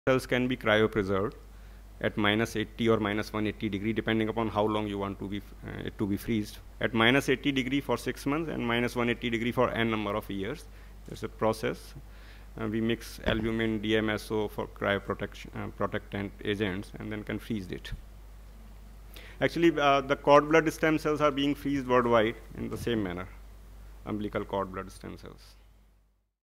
Dr. Narendra Agrawal- Speech2-Blood Stem Cell Transplant process
Dr. Narendra Agrawal, Consultant, Hemoto-Oncology and Blood Marrow Transplant Unit, RGCI & Research Institute, New Delhi talks about bone marrow transplant process.